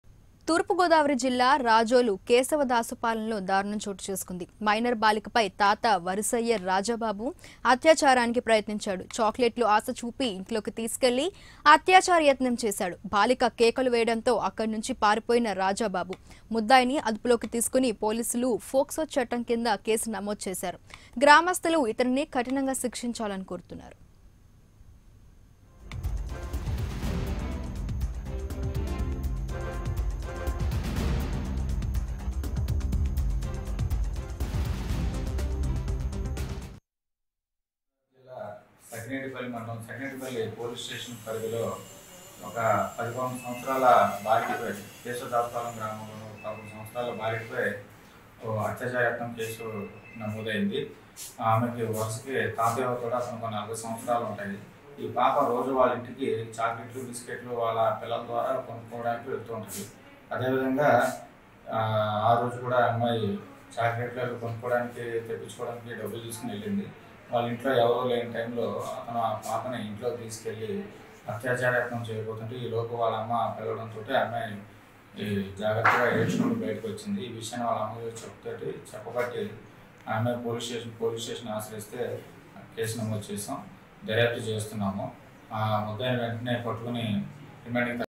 Minor Girl Assaulted by Grandfather In Razole, East Godavari | Prime9 News
Minor Girl Sexually Assaulted by Grandfather In Razole, East Godavari | Prime9 News